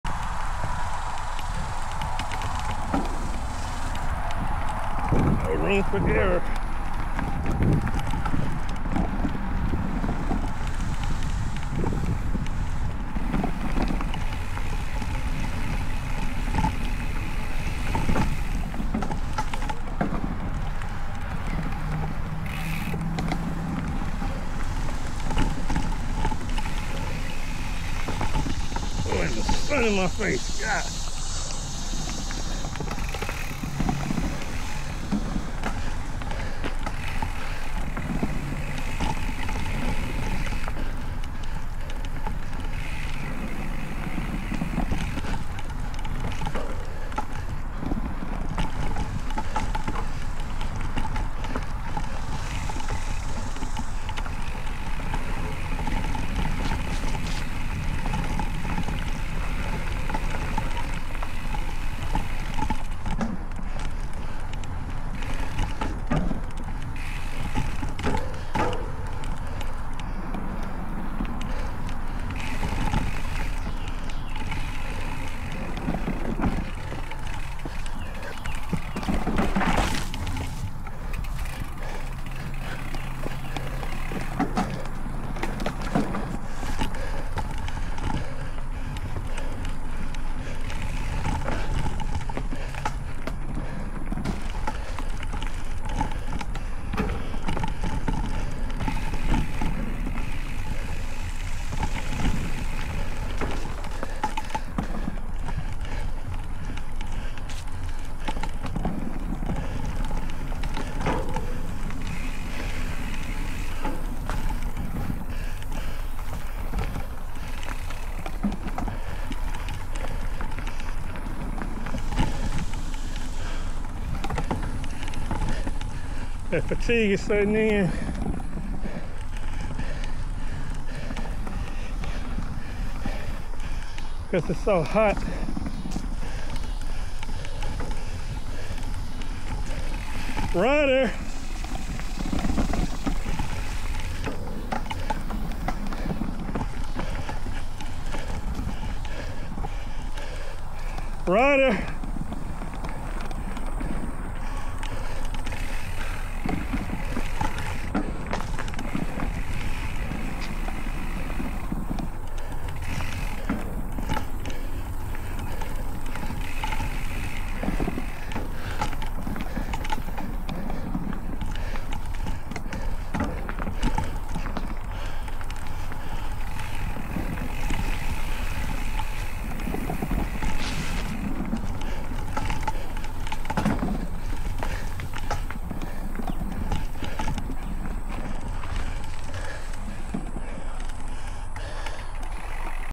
Insta360 X4-Swamp Thing Trail 2024 360
Short ride along the swamp banks of Oyster Creek.